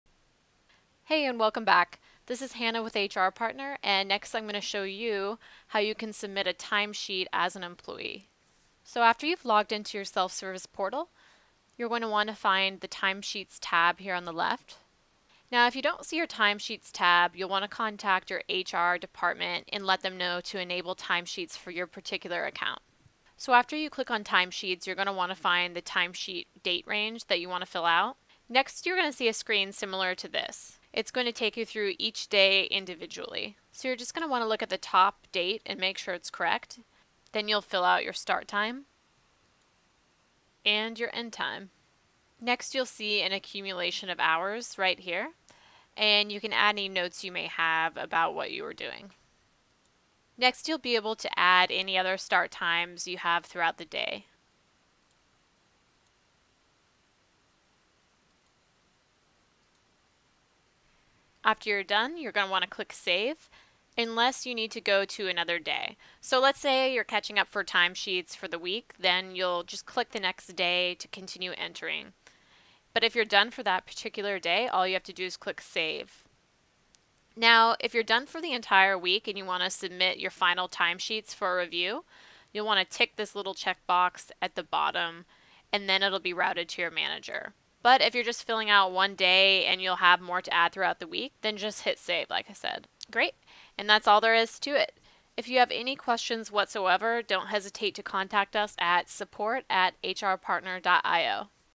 Timesheets for Employees Tutorial - HR Partner
A tutorial on how employees can fill out timesheets within HR Partner.